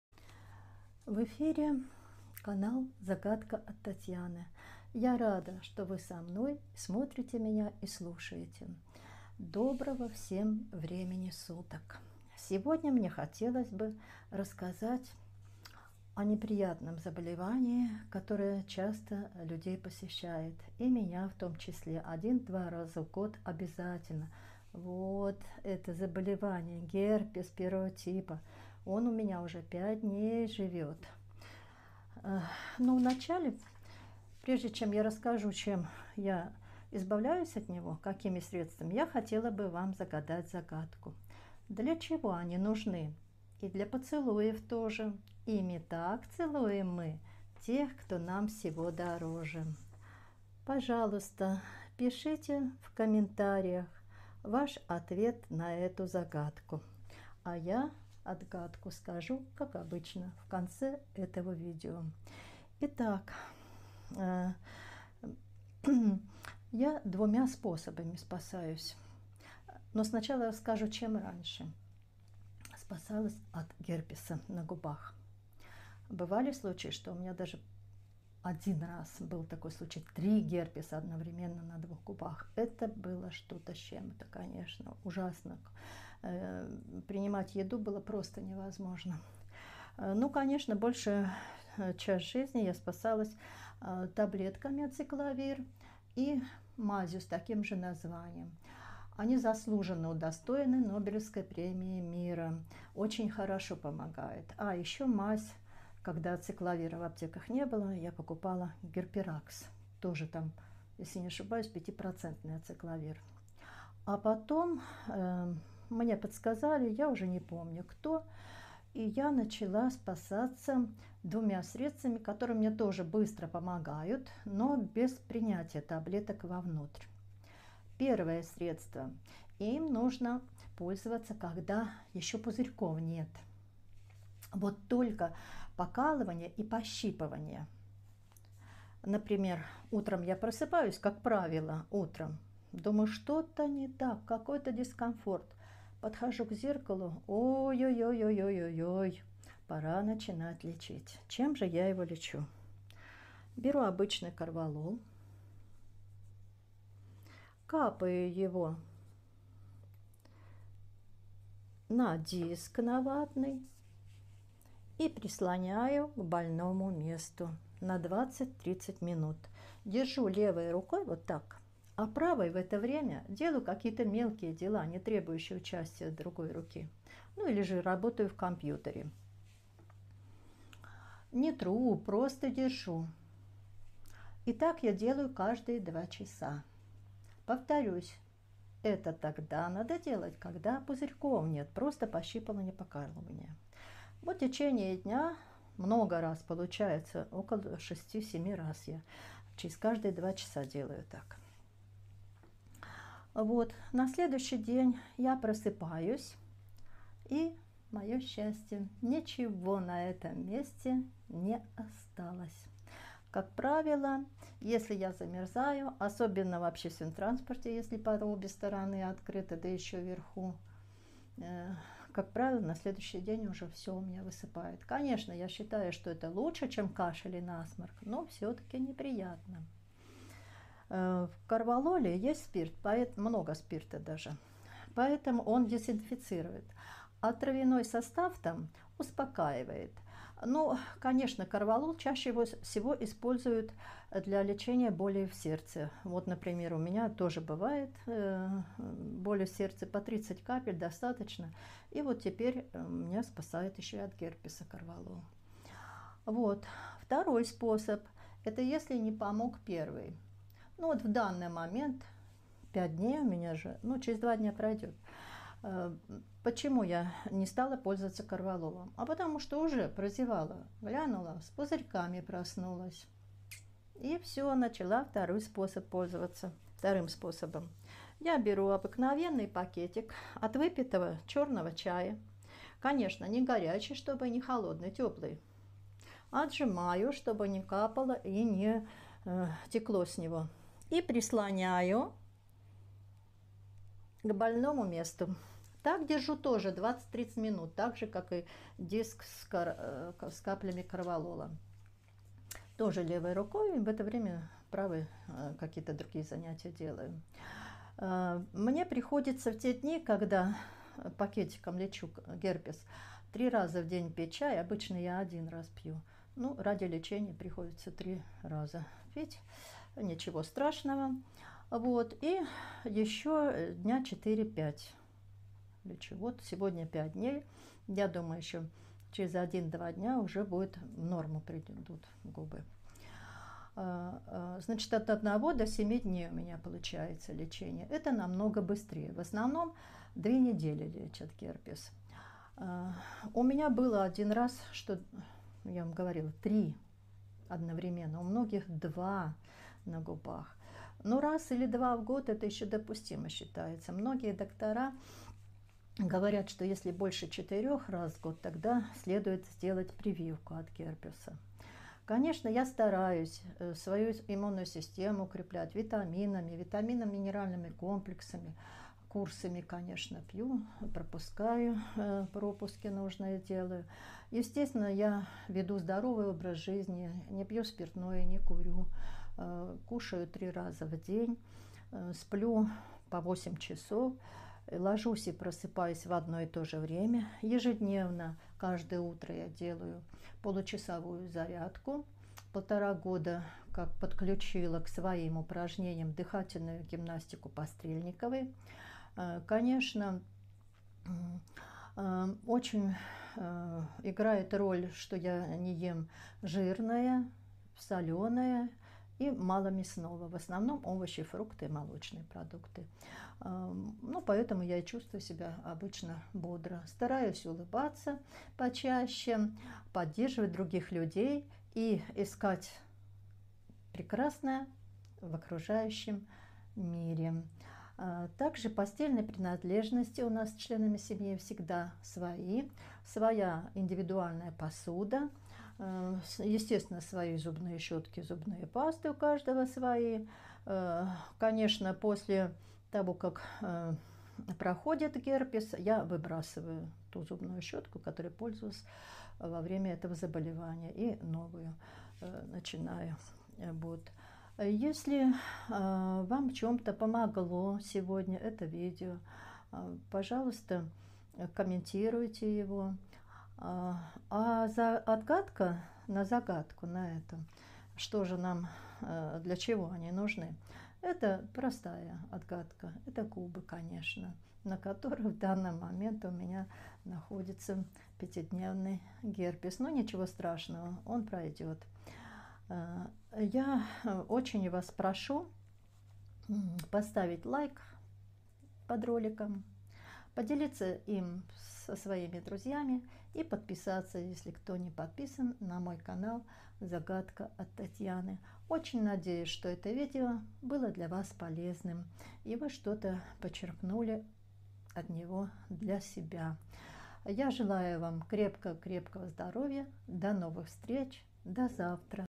герпесметодылечения Герпес на губах. Лечу за 1-7 дней.
Всем здравствуйте! Сегодня в видео я расскажу и покажу, как и чем я лечусь от герпеса на губах. Эти два быстрейшие способы избавления от неприятного заболевания я очень много лет делала на себе, а теперь решила показать вам.